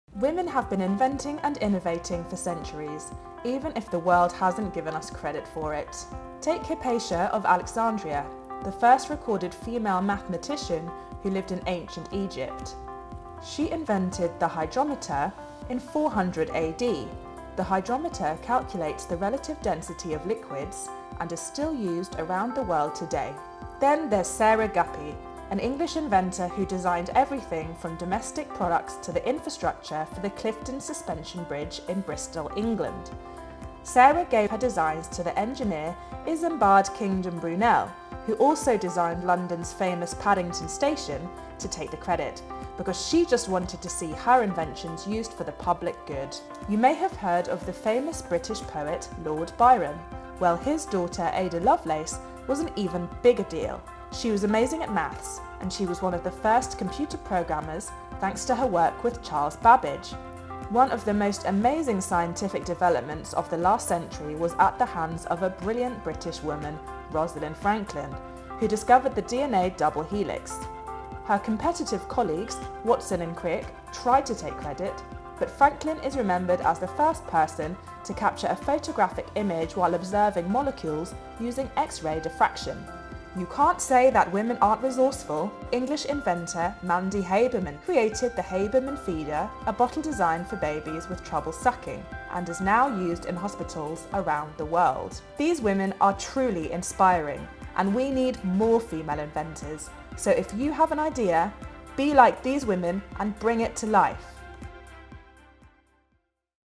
Pioneering female inventors you haven’t heard of but need to know!
Women have been inventing and innovating since the dawn of time! This BritishScienceWeek get inspired by these amazing female pioneers and start inventing yourself!